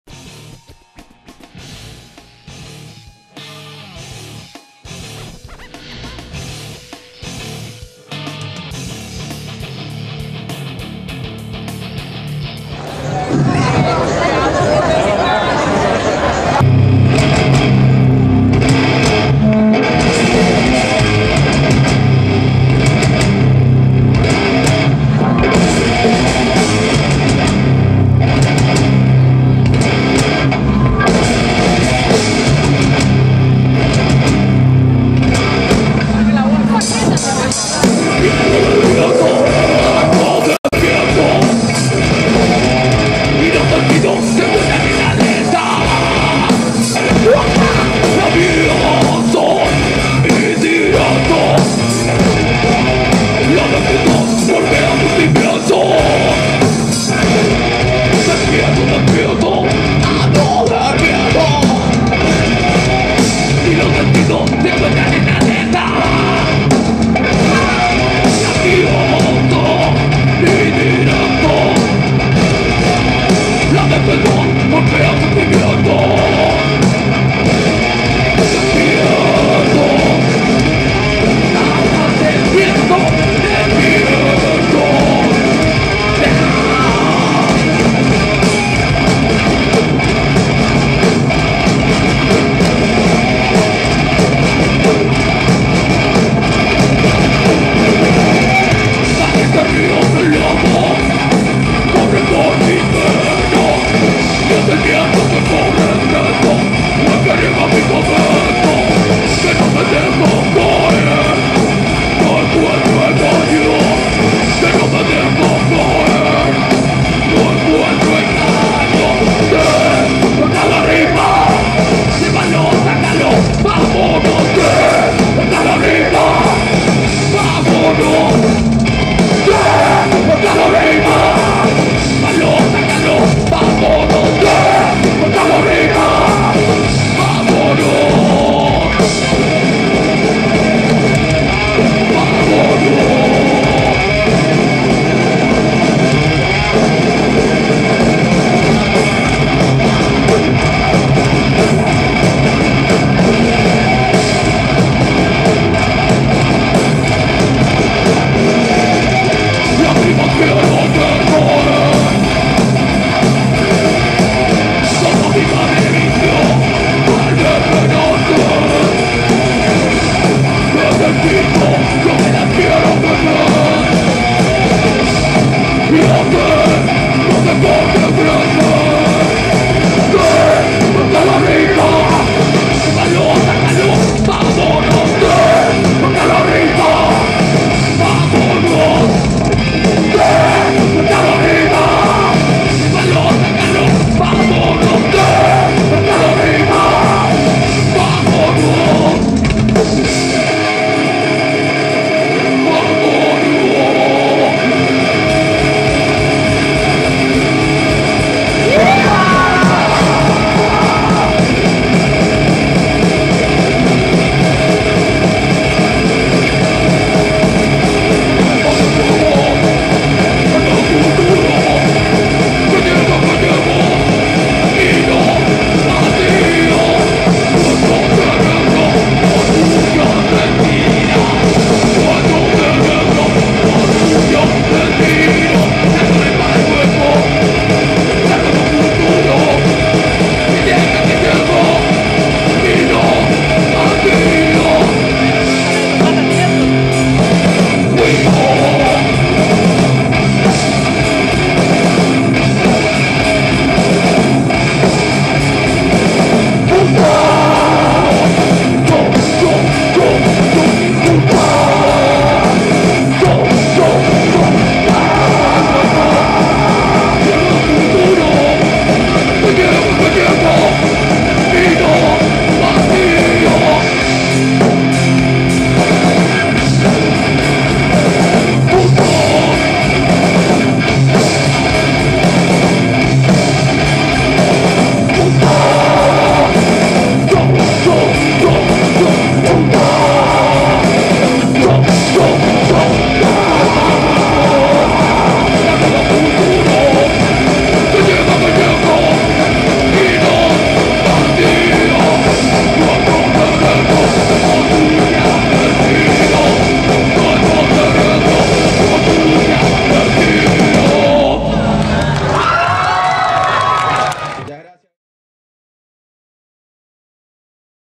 Tribu en Yaiza, Lanzarote 2006
Tema en directo de Tribu en un concierto dado en Yaiza, Lanzarote en el año 2006